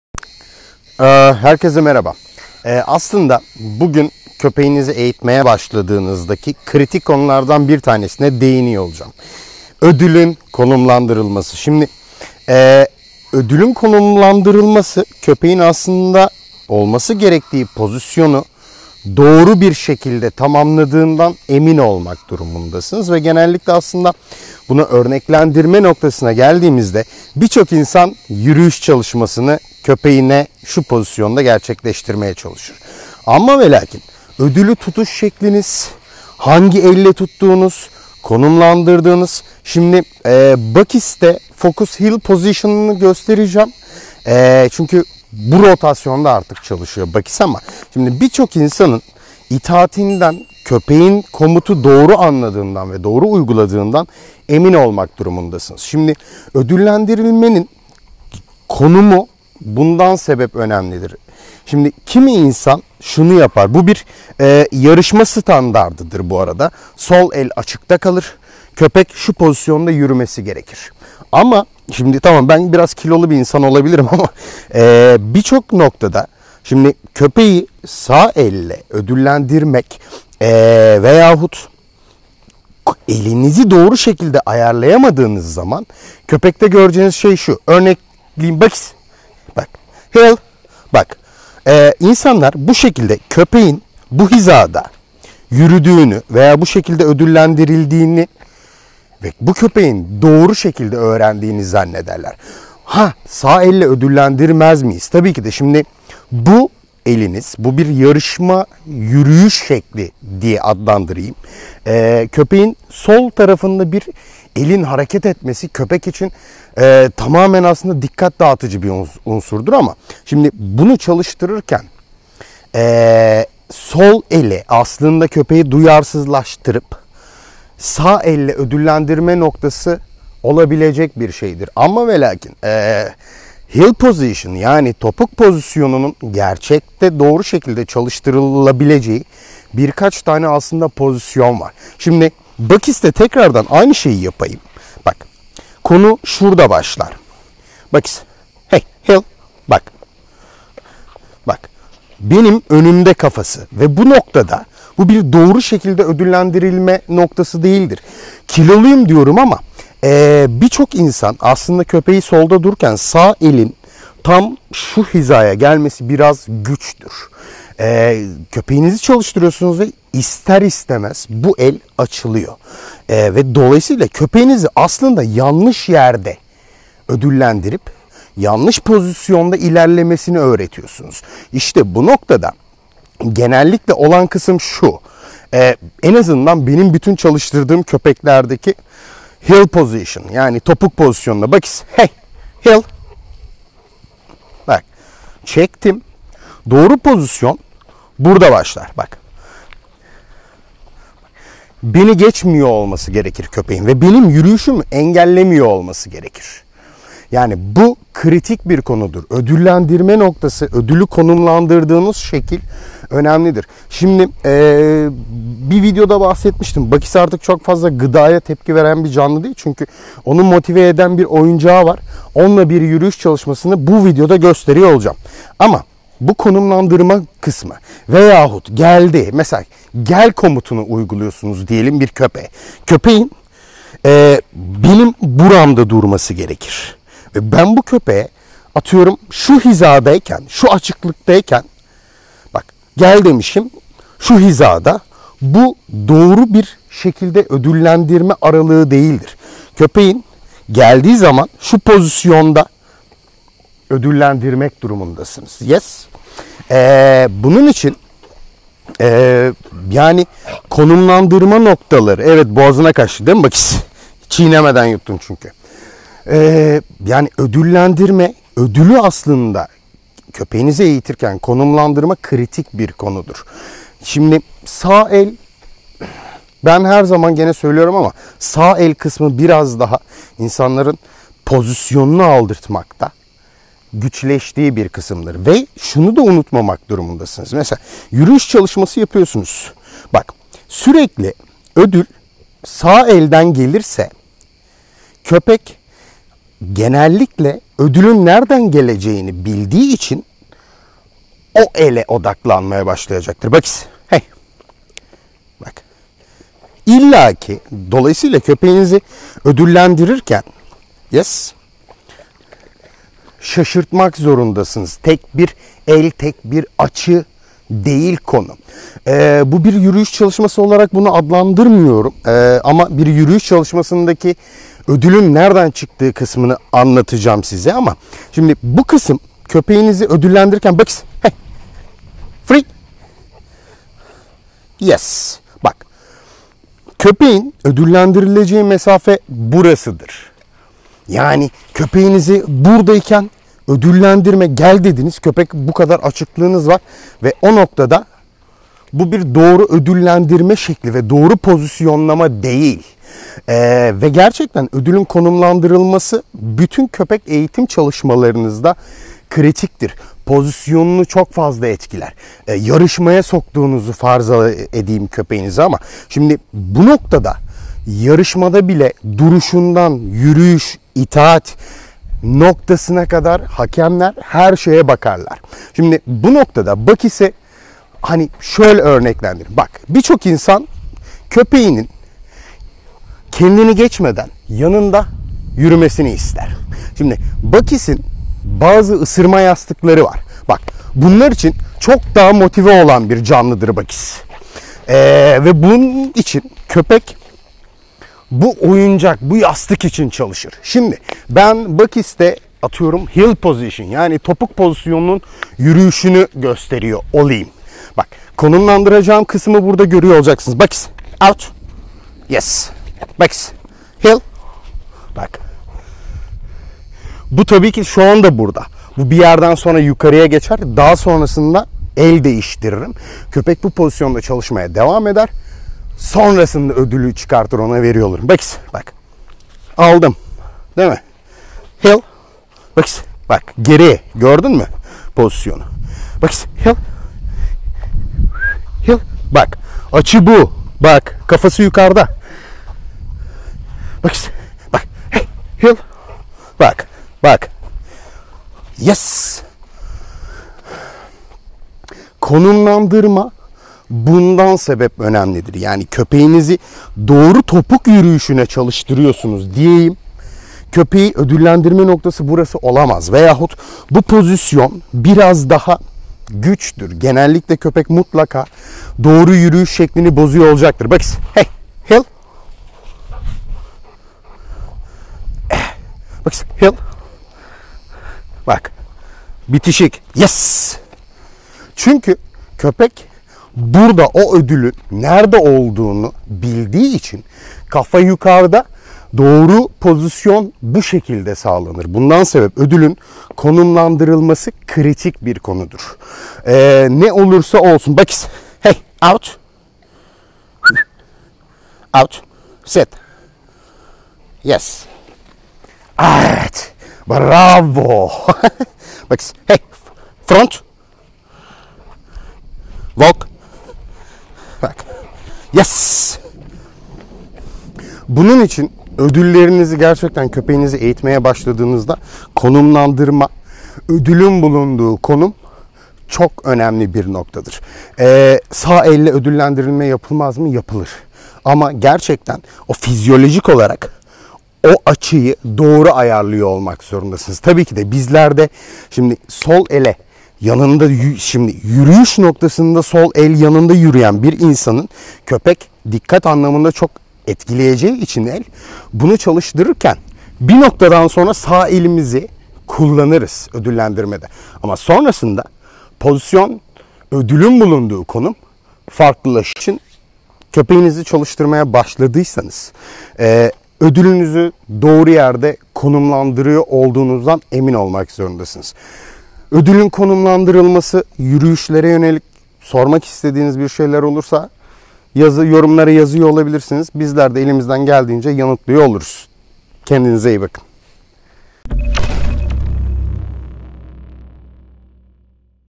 KÖPEK EĞİTİMİNDE ÖDÜLÜN KONUMLANDIRILMASI (CANE CORSO EĞİTİMİ)
Köpeğinizi eğitirken kullanılan ödülün doğru konumlandırılması ve doğru pozisyonda ödüllendirilmesi önemlidir. Bütün köpek ırkları için geçerli olan bu konu heleki CANE CORSO, ROTTWEILER, GERMAN SHEPHERD ve daha bir çok köpeğin eğitim sürecinde doğru pozisyon için eğitilmesinde kritiktir. Köpeğin eğitimden yanlış pozisyonda veya açıda ödüllendirmek köpeğin itaat çalışmalarını sekteye uğratır.